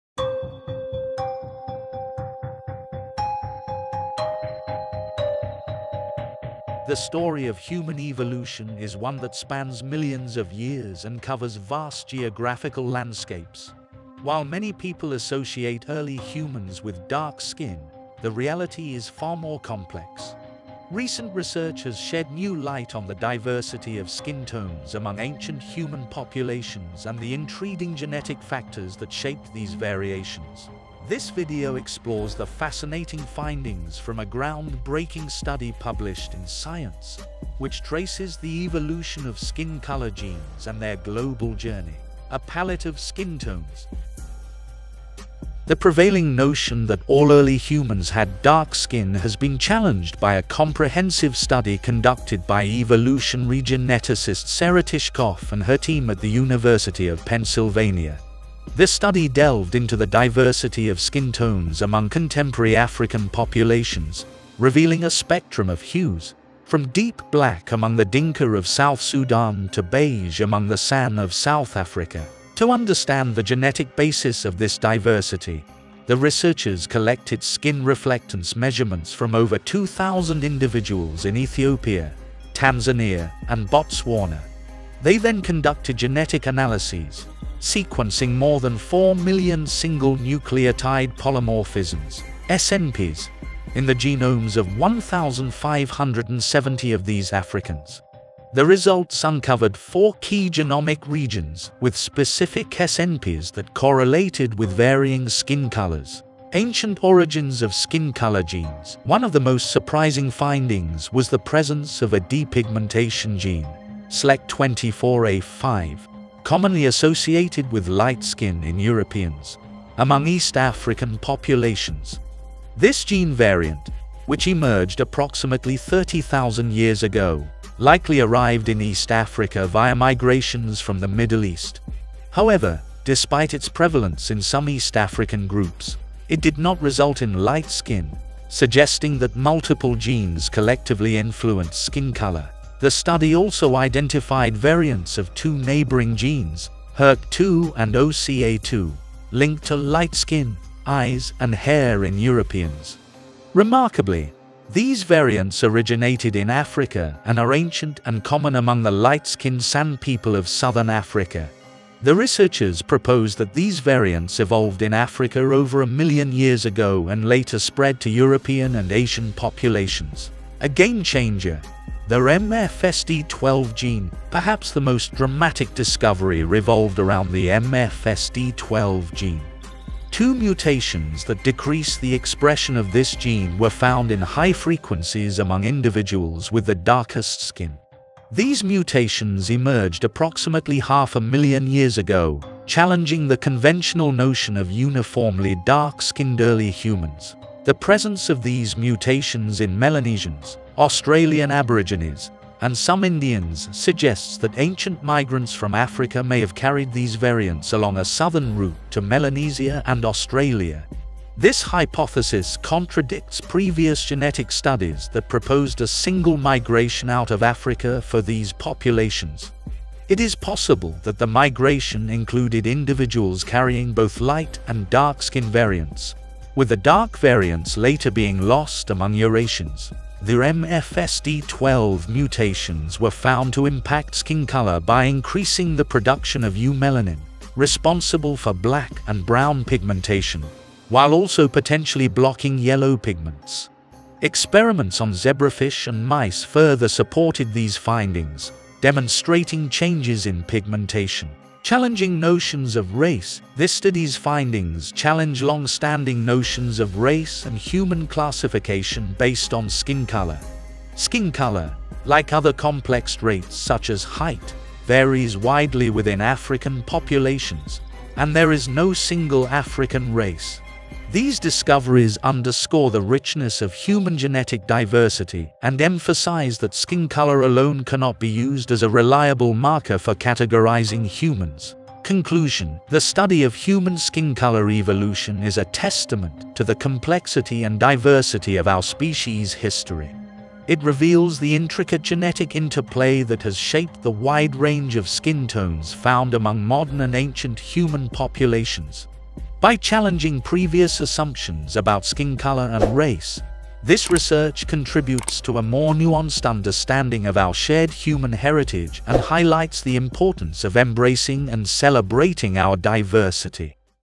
Were the first humans black?
This video explores the fascinating findings from a groundbreaking study published in Science, which traces the evolution of skin color genes and their global journey.

Join our free history community for insights, discussions, hidden knowledge and more